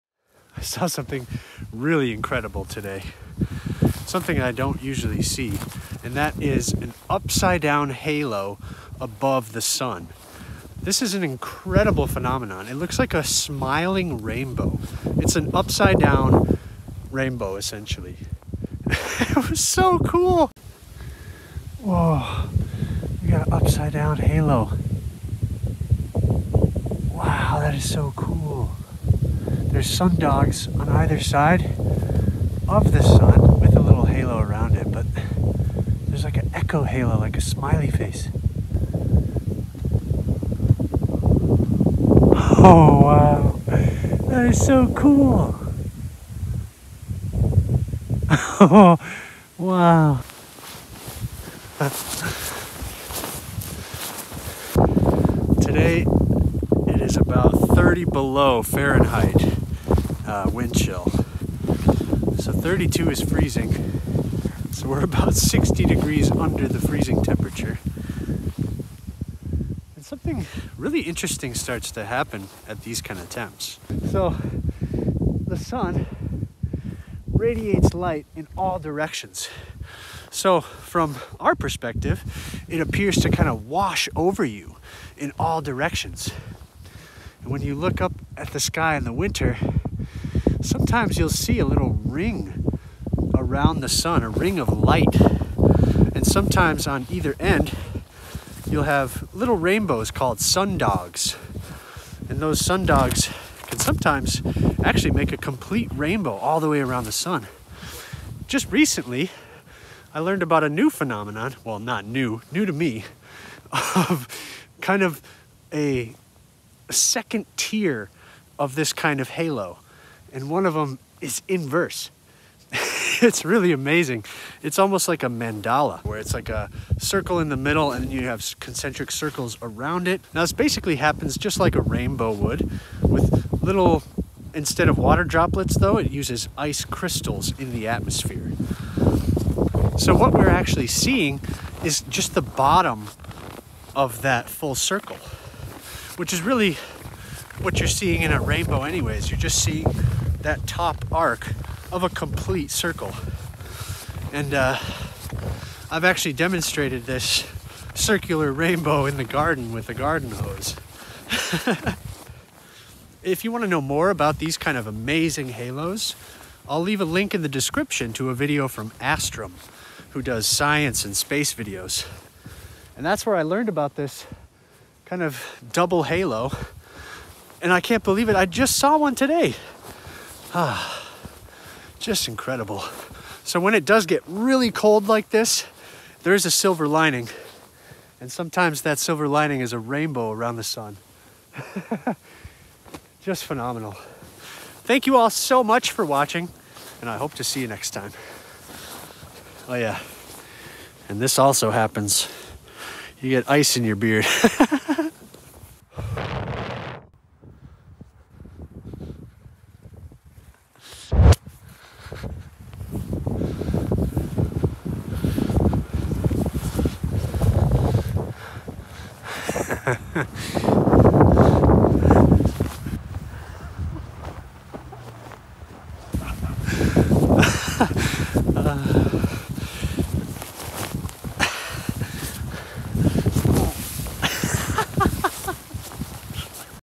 🌈Upside-Down Rainbow!?!
On a cold Winter day in Minnesota, with -30°F wind-chill, I spot an ice halo around the sun with sundogs and a circumzenithal arc (upside-down rainbow)!